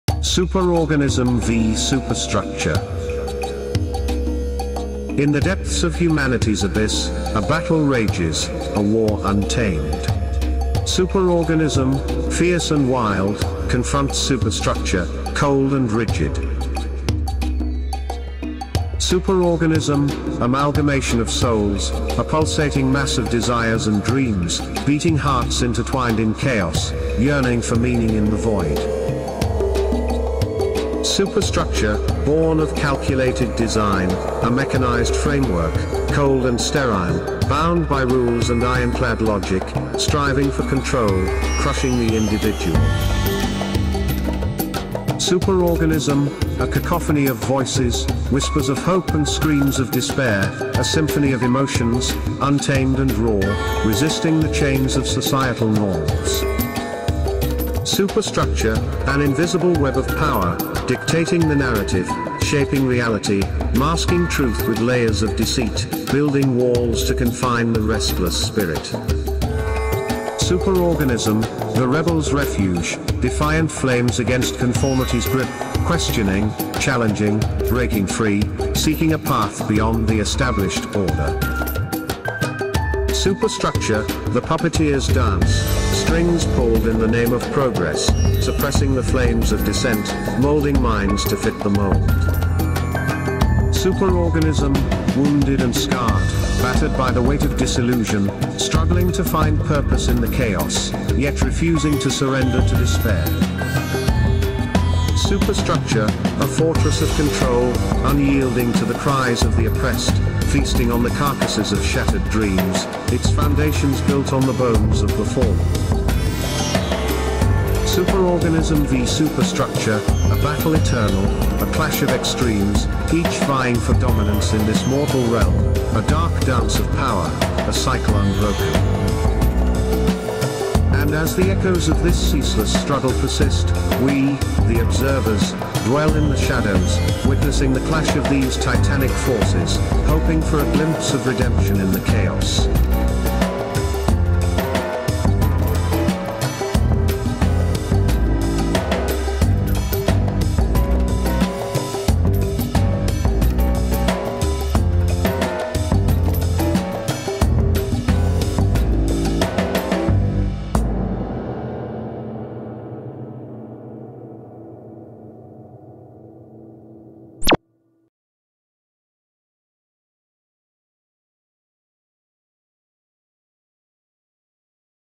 Superorganism v Superstructure | Original Intense Music | A.I. Art and Poetry
Superorganism v Superstructure was written by A.I. using ChatGPT 4.0. The associated images were created using A.I. at NightCafe.studio. The accompanying music is by Theo Michelfeld.

"Superorganism v Superstructure" by ChatGPT 4.0

In the depths of humanity's abyss,
a battle rages, a war untamed.
Superorganism, fierce and wild,
confronts Superstructure, cold and rigid.

Superorganism, amalgamation of souls,
a pulsating mass of desires and dreams,
beating hearts intertwined in chaos,
yearning for meaning in the void.

Superstructure, born of calculated design,
a mechanized framework, cold and sterile,
bound by rules and ironclad logic,
striving for control, crushing the individual.

Superorganism, a cacophony of voices,
whispers of hope and screams of despair,
a symphony of emotions, untamed and raw,
resisting the chains of societal norms.

Superstructure, an invisible web of power,
dictating the narrative, shaping reality,
masking truth with layers of deceit,
building walls to confine the restless spirit.

Superorganism, the rebel's refuge,
defiant flames against conformity's grip,
questioning, challenging, breaking free,
seeking a path beyond the established order.

Superstructure, the puppeteer's dance,
strings pulled in the name of progress,
suppressing the flames of dissent,
molding minds to fit the mold.

Superorganism, wounded and scarred,
battered by the weight of disillusion,
struggling to find purpose in the chaos,
yet refusing to surrender to despair.

Superstructure, a fortress of control,
unyielding to the cries of the oppressed,
feasting on the carcasses of shattered dreams,
its foundations built on the bones of the fallen.

Superorganism v Superstructure,
a battle eternal, a clash of extremes,
each vying for dominance in this mortal realm,
a dark dance of power, a cycle unbroken.

And as the echoes of this ceaseless struggle persist,
we, the observers, dwell in the shadows,
witnessing the clash of these titanic forces,
hoping for a glimpse of redemption in the chaos.